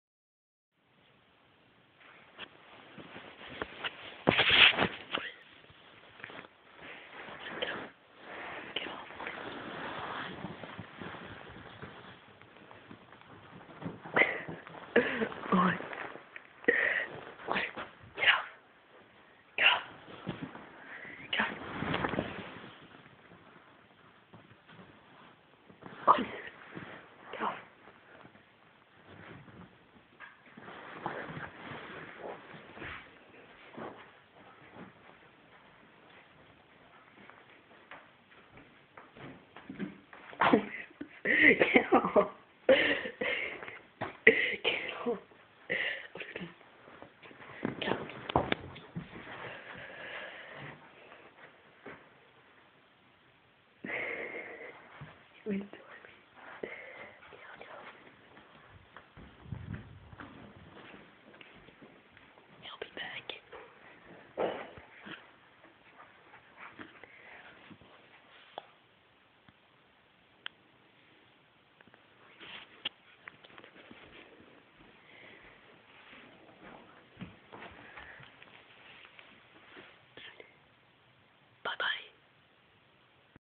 Cutest dog ever
Otis is a very smart dog haha